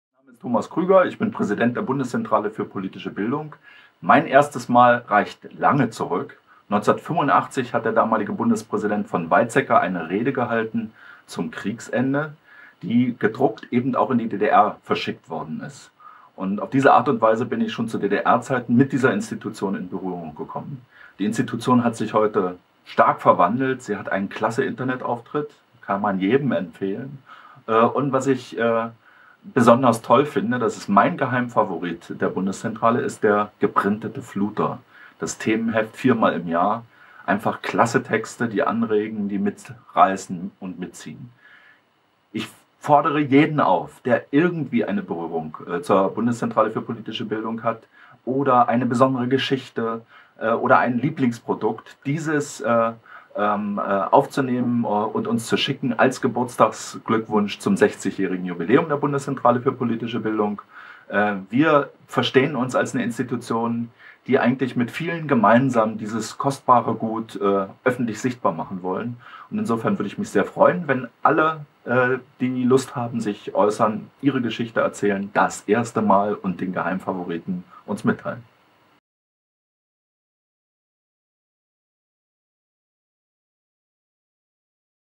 bpb-Präsident Thomas Krüger freut sich über Ihre Glückwünsche!
Thomas Krüger, Präsident der Bundeszentrale für politische Bildung/bpb, wünscht sich so viele Kommentare und Beiträge wie möglich zum 60. Geburtstag. Sein Lieblingsprodukt: der fluter. Was ist Ihres? Wann sind Sie zum ersten Mal mit Produkten der bpb in Berührung gekommen? Was wünschen Sie der bpb zum 60.? Laden Sie jetzt Ihr Video bei YouTube hoch und erzählen sie uns Ihre Geschichte!